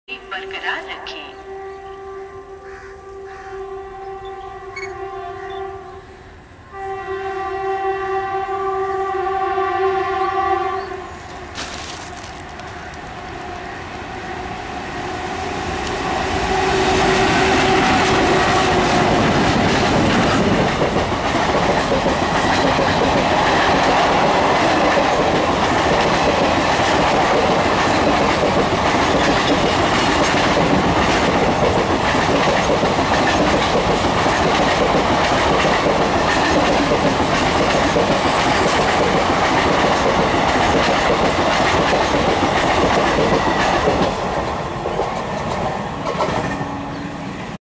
Amazing triplets!! WAG-5E + Twin WDM-2 sprints away with empty Parcel Express!!
Powered by triplets 23636 BZA WAG- 5E, 17852 and 17734 WDM- 2 Twins of KGP (Kharagpur) shed in two different liveries, an empty 22 coaches HCPV (Parcel) Freight sprints past Sankrail station through the up main line. Sankrail is situated on SER's Howrah- Kharagpur stretch.